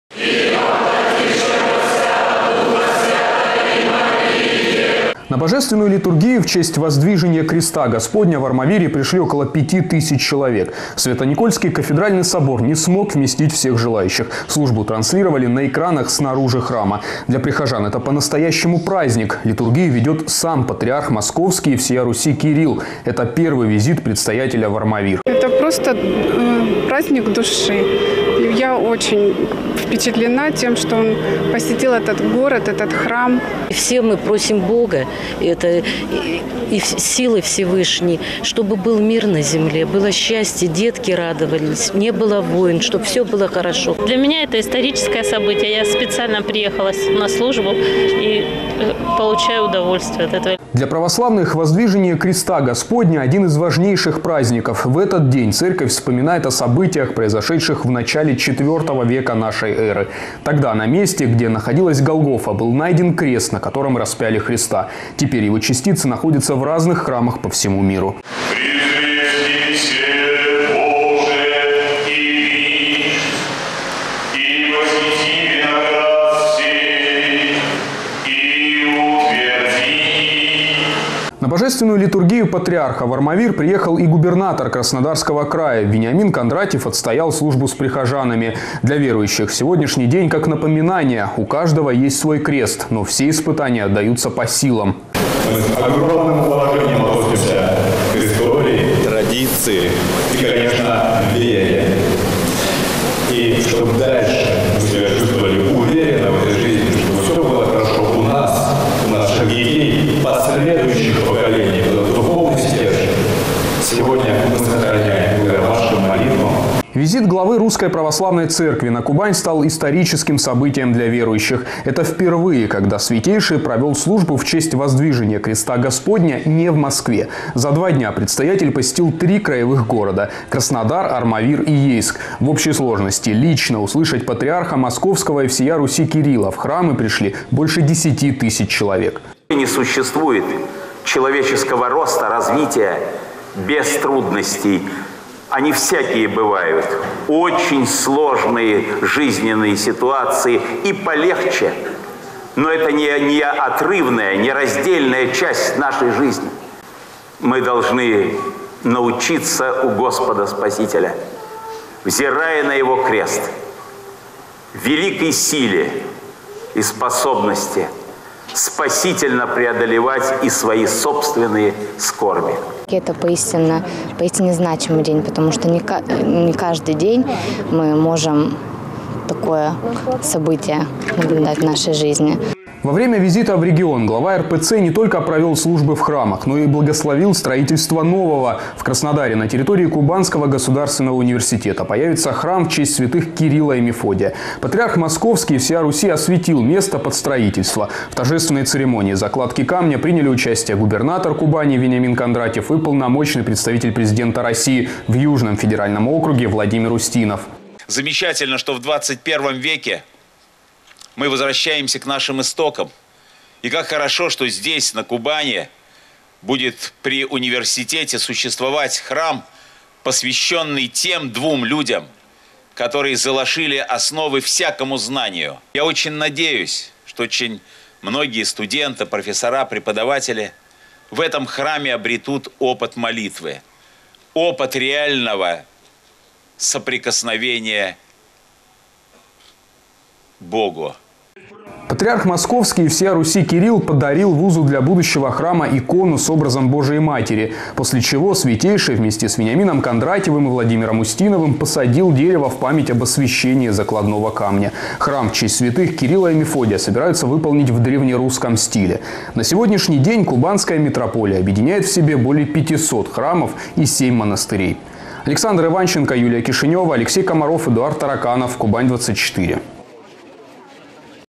Патриарх Кирилл провел в Армавире литургию в честь великого православного праздника. Эфкате
Воздвижение креста Господня сегодня празднуют православные христиане. Для Кубани этот день стал историческим. Провести Божественную литургию в храмах Краснодарского края лично приехал Патриарх Московский и всея Руси Кирилл.

Только в Армавире на службу пришли тысячи прихожан. На Божественную литургию Патриарха в Армавир приехал и губернатор Краснодарского края. Вениамин Кондратьев. На божественную литургию в честь Воздвижения креста Господня в Армавире пришли около 5 тысяч человек. Свято-Никольский кафедральный собор не смог вместить всех желающих. Службу транслировали на экранах снаружи храма. Для прихожан это по-настоящему праздник - литургию провел сам Патриарх Московский и всея Руси Кирилл. Это первый визит предстоятеля в Армавир.

Для православных Воздвиже